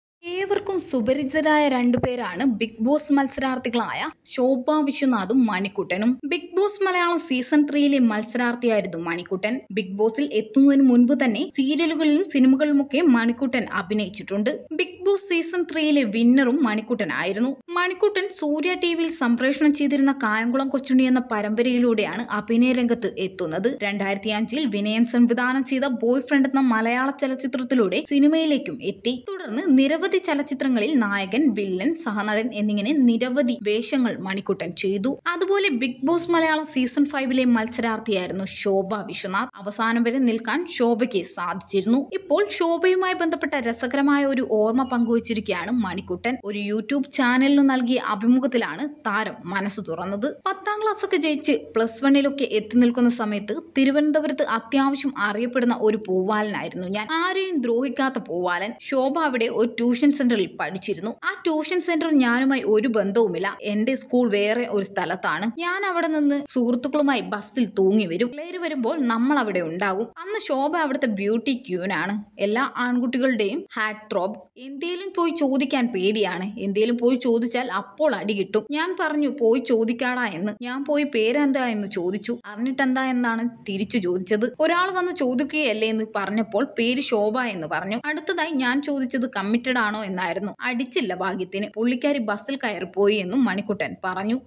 ഞാൻ തിരുവനന്തപുരത്തെ വലിയ പൂവാലനായിരുന്നു, ശോഭ അവിടത്തെ ബ്യൂട്ടി ക്വീനും : മണിക്കുട്ടൻ
ഞാൻ തിരുവനന്തപുരത്തെ വലിയ പൂവാലനായിരുന്നു, ശോഭ അവിടത്തെ ബ്യൂട്ടി ക്വീനും : മണിക്കുട്ടൻ manikkuttan shobhaviswanath bigboss bb3 bb5 malayalambb actor enterpreneur third eye news